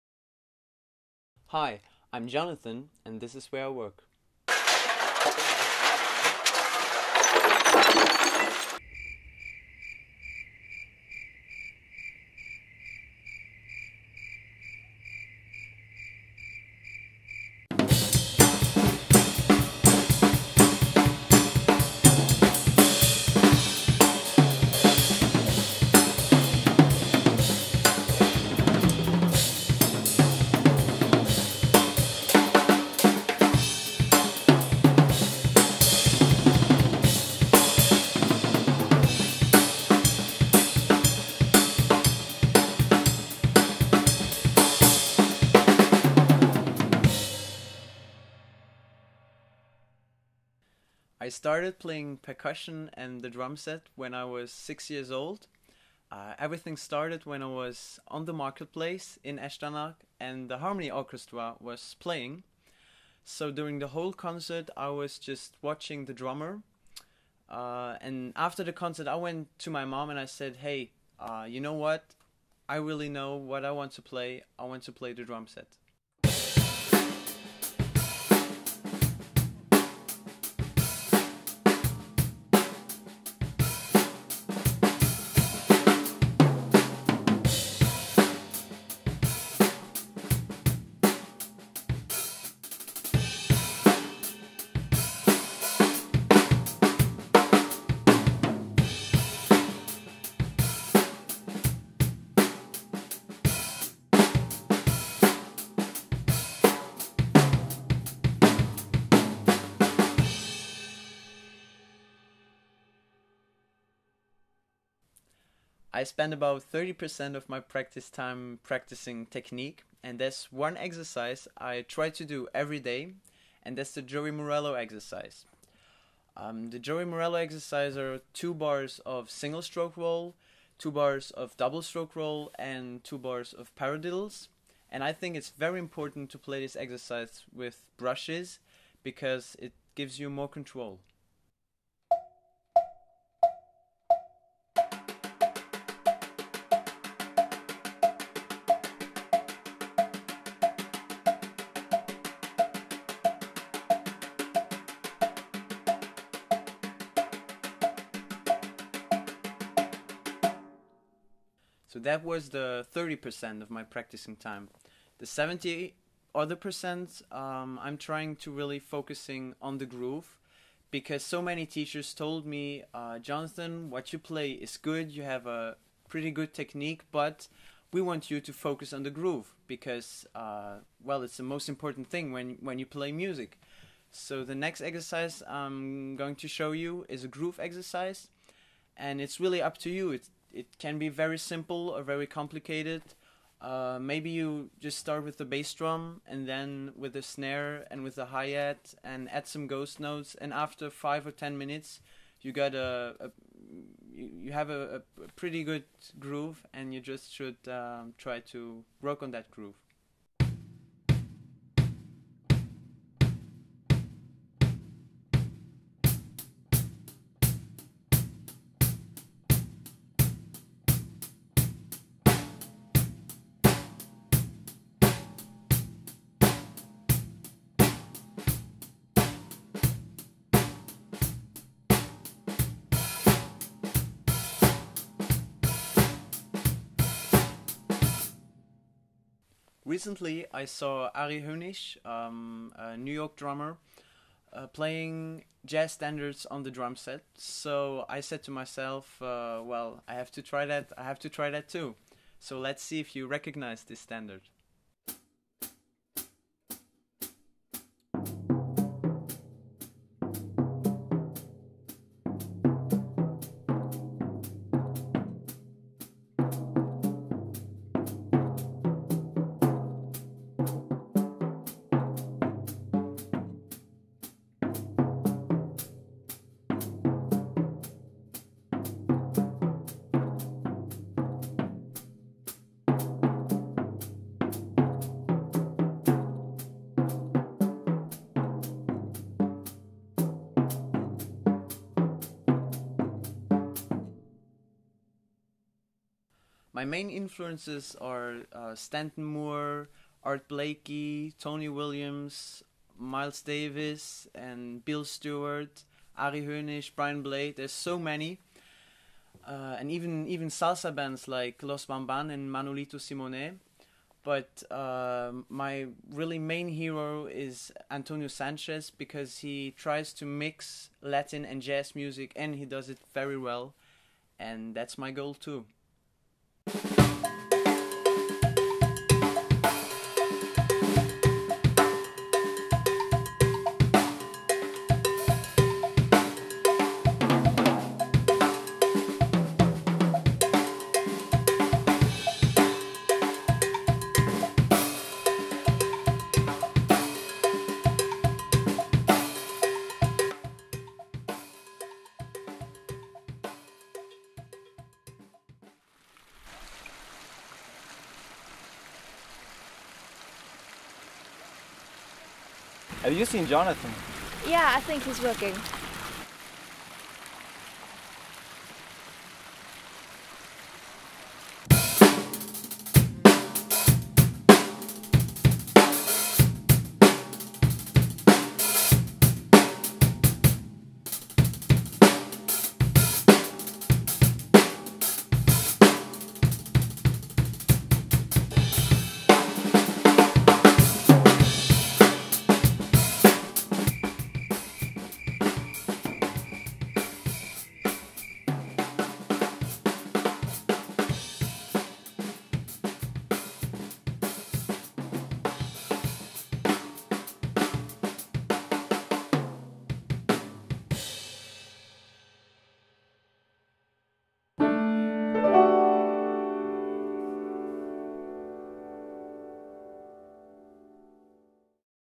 "This Is Where I Work" Episode IV: Jonathan Levi
Written & Produced by Monique Hinkel and George Letellier for MGL Studio Productions.

In this episode you are introduced to the amazing up-and-coming drummer and percussionist Jonathan Levi. At 15 years old he is surely one to keep a close eye on now and in the future.

Thanks to Myrna, David and Pierre for their cameo appearances.